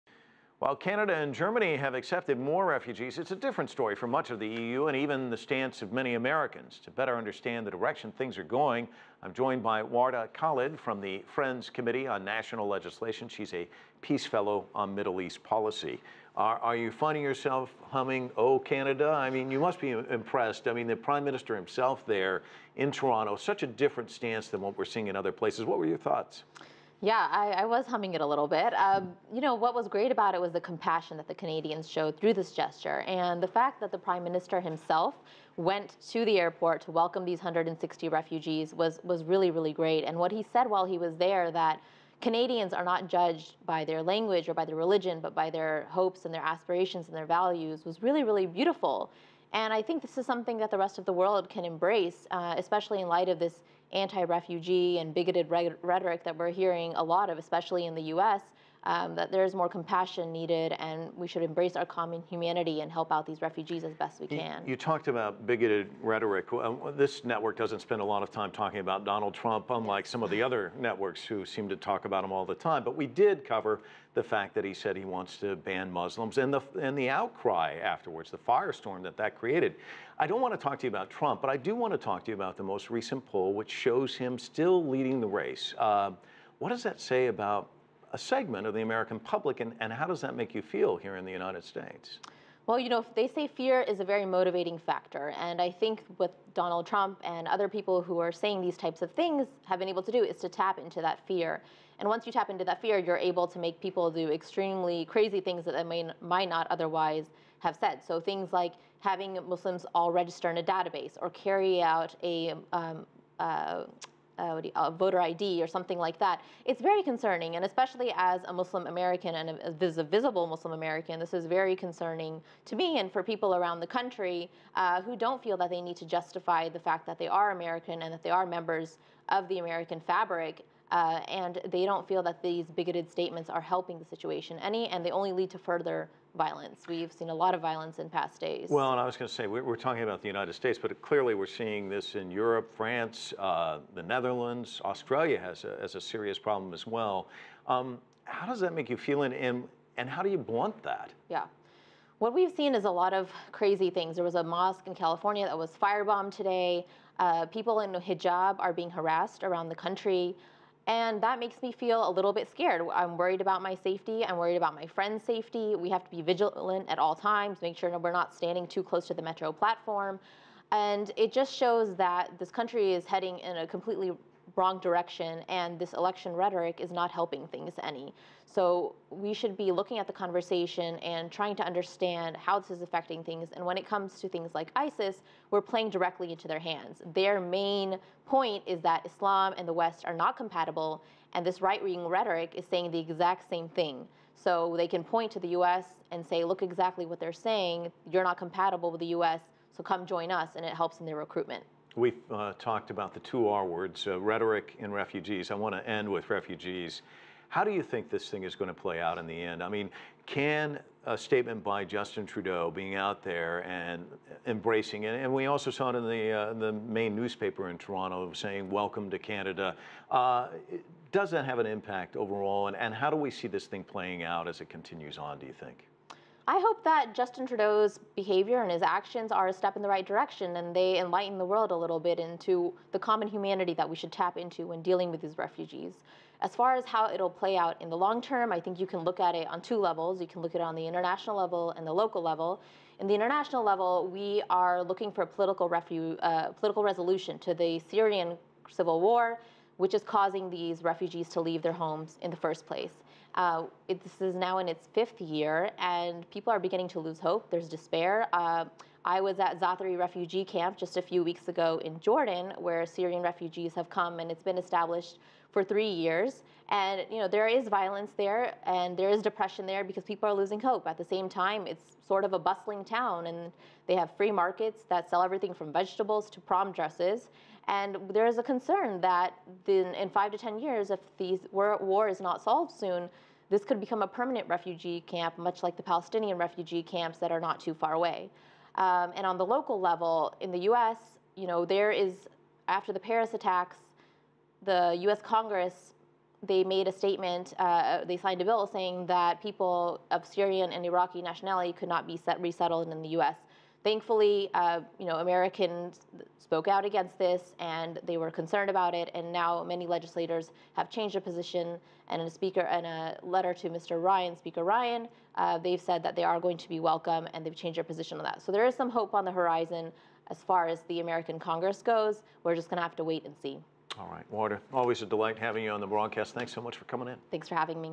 Wardah Khalid on Canada's response to refugees
CCTV America's Elaine Reyes interviewed Wardah Khalid. She's a Peace Fellow on Middle East Policy.